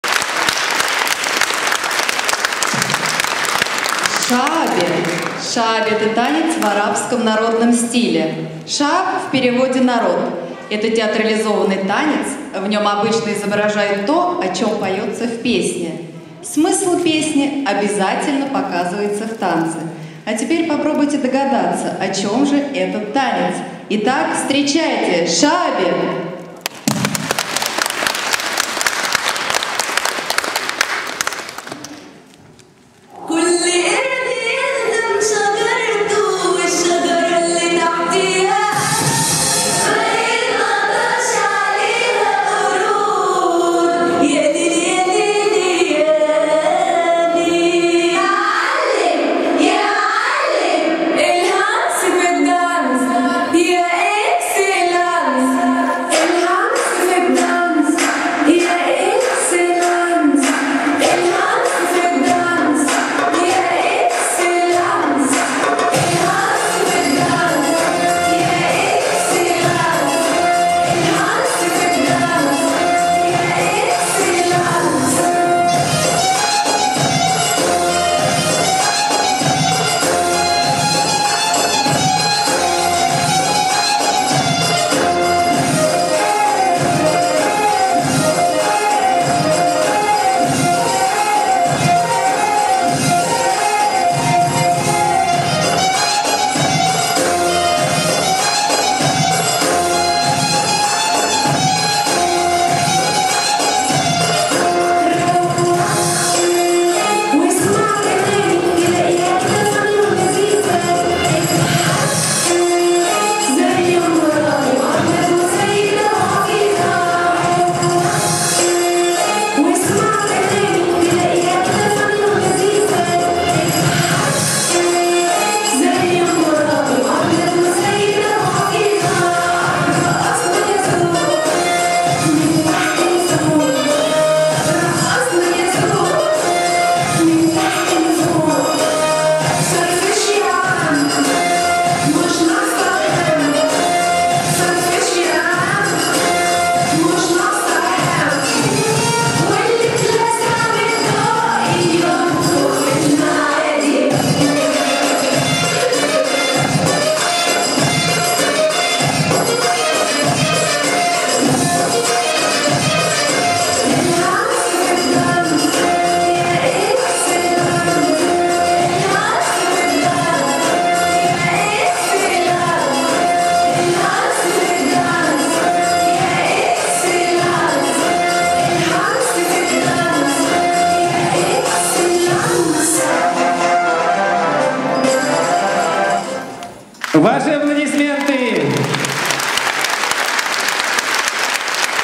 12 Шааби клуб "Сайёра".MOD
Отчетный концерт клуба"Сайёра" г Борисоглебск. 29 мая 2012 года